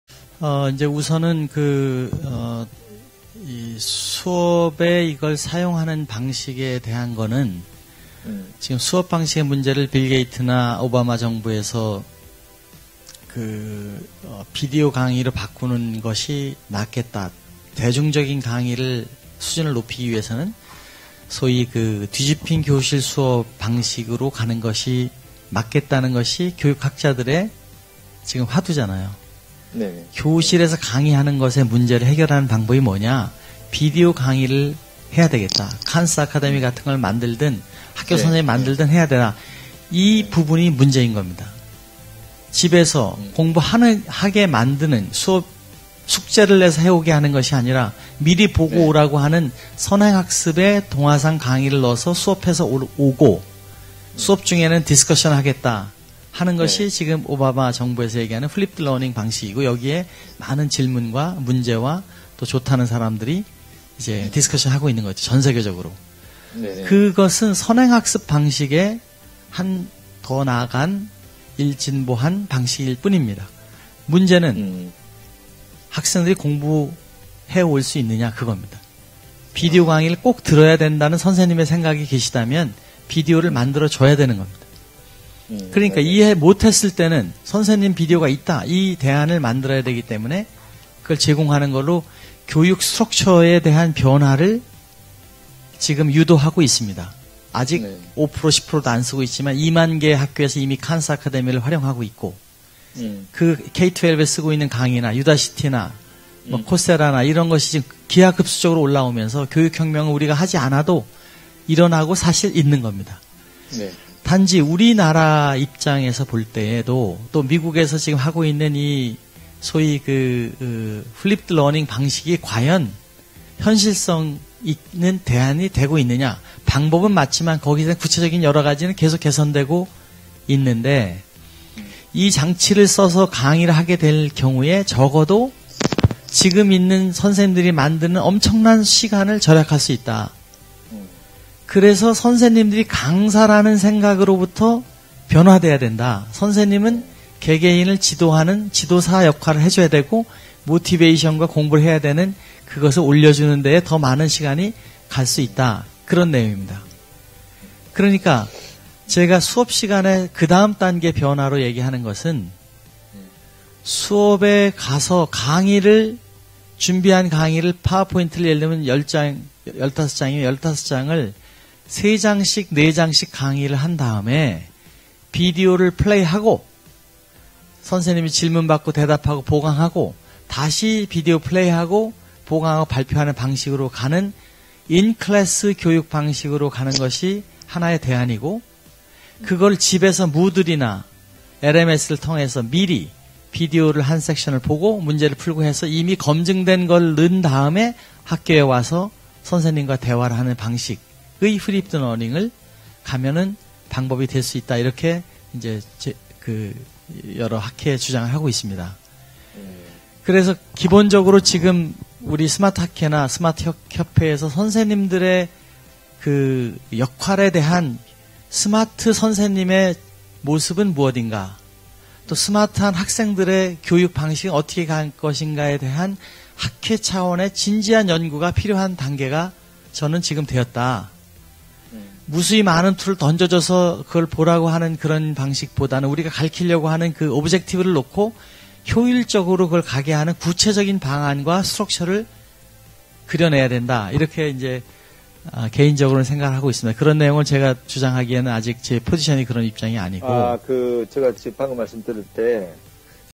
Flipped 교육에 비디오 강의를 집에서 보는 방식 - 교실서 토의하는.자기주도학습. 문제&방안
ㄷ뒤집힌 교실 Flipped learning 교육은 자기 주도 학습 방식으로 교실의 라이브 강의의 문제를 해결하여 각 학생들이 자신의 시간을 만들수 있는 방법이지만 강의가 비디오로 만드는 과정에 큰 병목의 문제가 있게 된다.  K-12에서 Khan의 비디오를 많은 학생들이 사용하는 것을 염두한 빌게이트의 생각등.. 뒤집힌 교실은 아름다운 혁명은 한단계 더나가 교실에서 수업- inclass education 으로 명명할수 있는 새 iStudio 방식으로도 발전될수 있다는 소견이다.